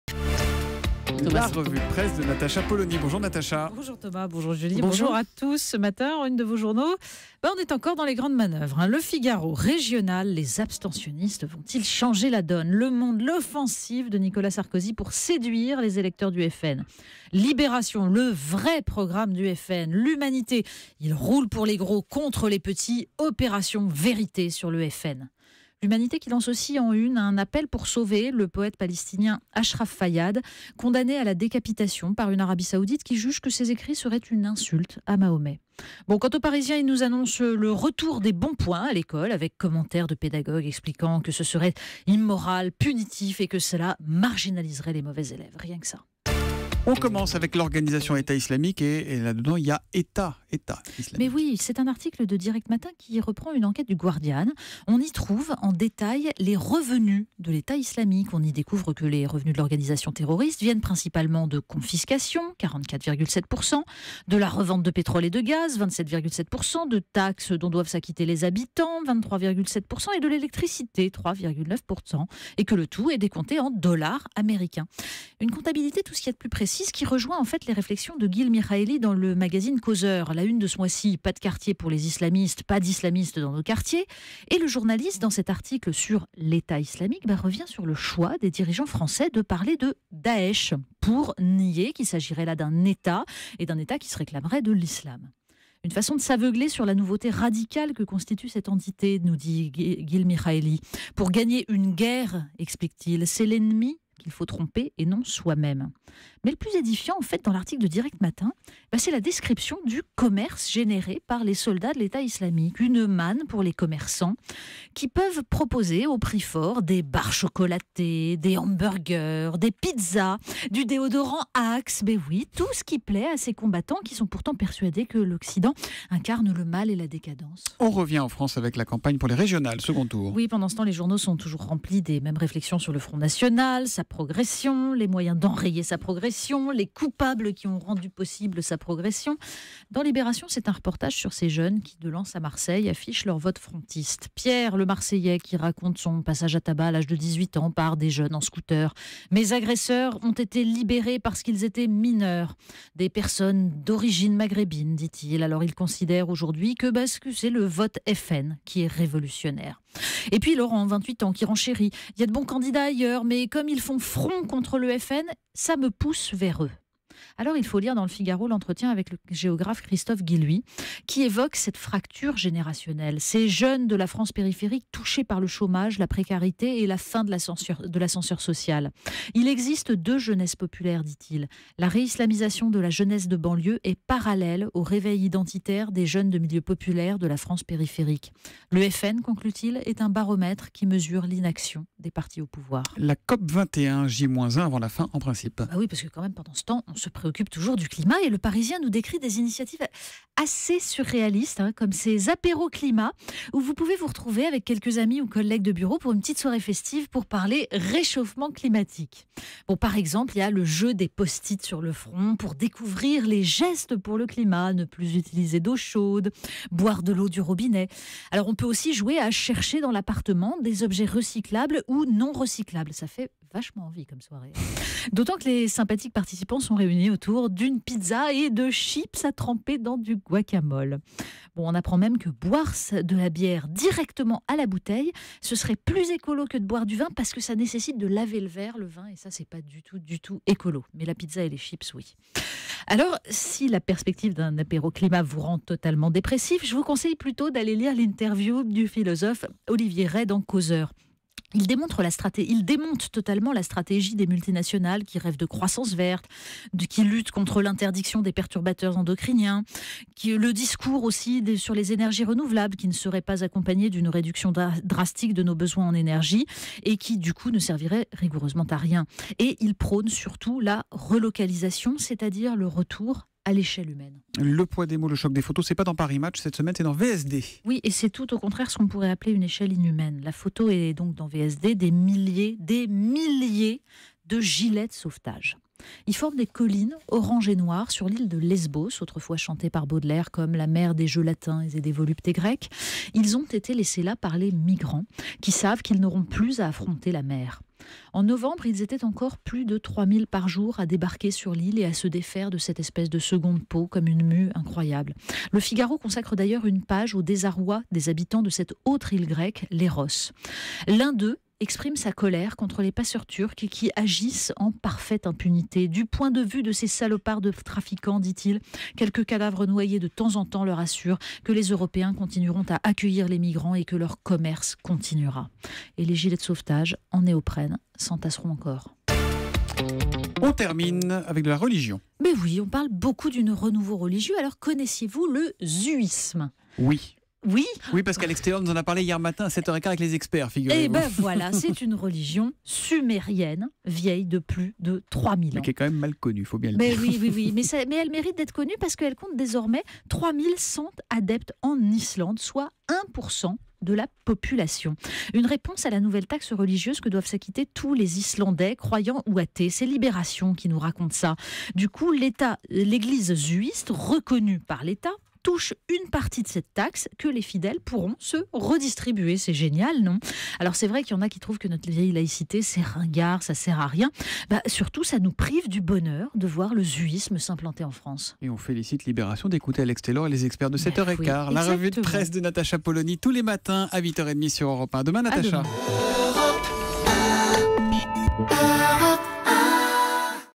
Régionales : les abstentionnistes vont-ils changer la donne
La presse quotidienne revient une nouvelle fois ce jeudi sur l'entre-deux tours des élections régionales.